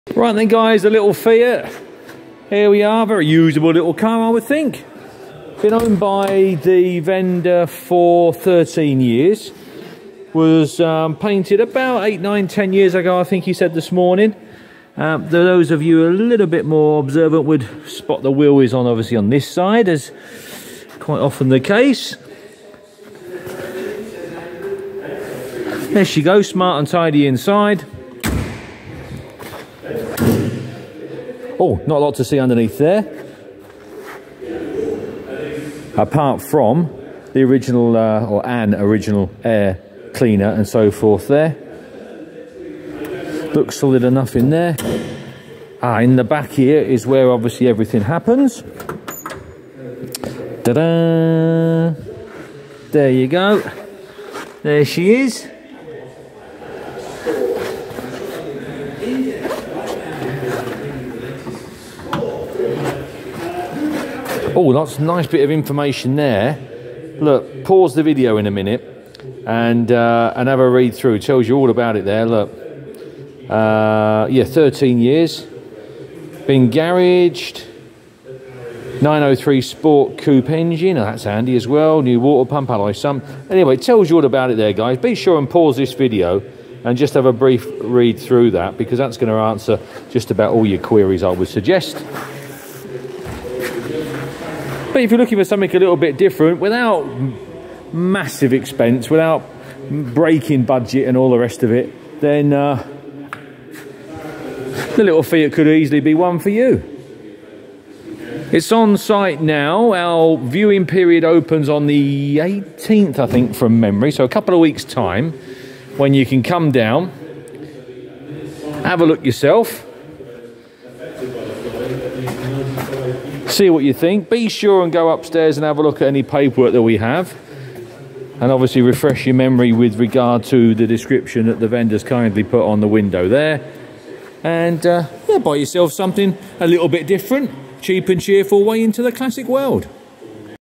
1964 FIAT 850 SALOON | MATHEWSONS CLASSIC CARS | AUCTION: 27, 28 & NOVEMBER 2024
1964 FIAT 850 SALOON | MATHEWSONS CLASSIC CARS | AUCTION: 27, 28 & NOVEMBER 2024

Join Derek, Dave, Paul and everyone at Mathewsons Classic Cars for our live online classic car, motorcycle and memorabilia auction on 27, 28 & 29 November 2024.